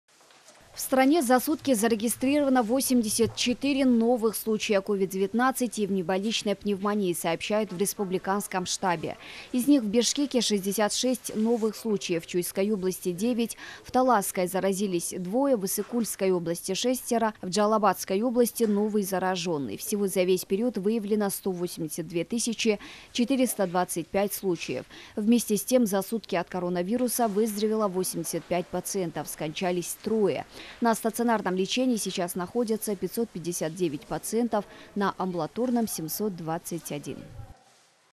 Статистика по COVID-19 в Кыргызстане за сутки: 84 новых случая, 3 летальных исхода
В стране за сутки зарегистрировано 84 новых случая COVID-19 и внебольничной пневмонии, сообщают в Республиканском штабе.

Всего за весь период выявлено 182 тыс. 425 случаев. Вместе с тем, за сутки от коронавируса выздоровело 85 пациентов, скончались трое. На стационарном лечении сейчас находятся 559 пациентов, на амбулаторном - 721.